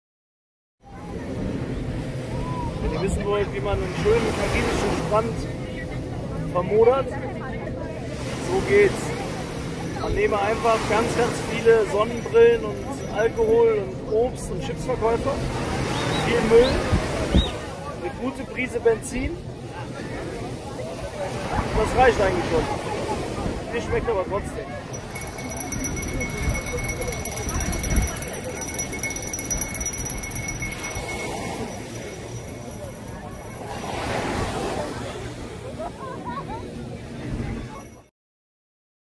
Wie man einen Traumstrand zerstört...
So macht man ein Paradies kaputt. Cartagena - Kolumbien.